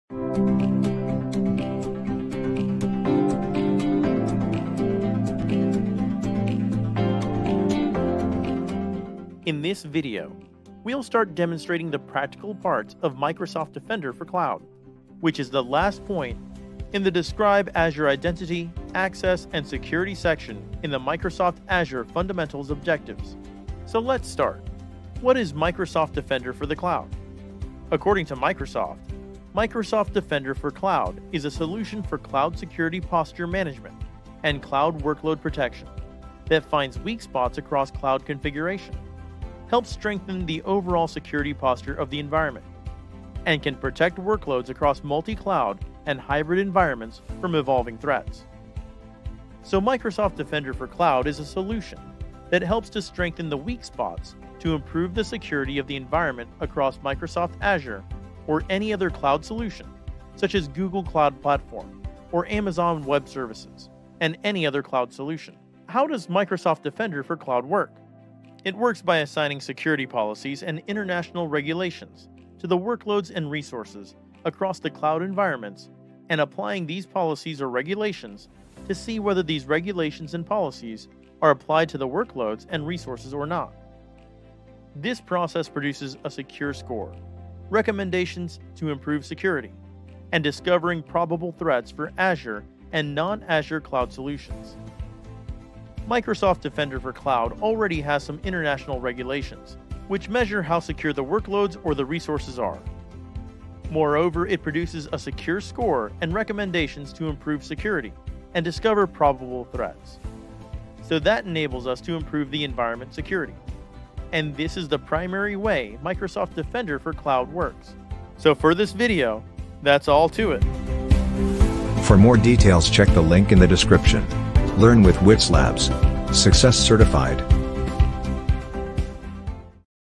What is Microsoft Defender for Cloud? | AZ-900 | Microsoft Azure Fundamentals | Whizlabs
Learn more about Microsoft Defender for Cloud by enrolling into AZ-900: Microsoft Azure Fundamentals certification course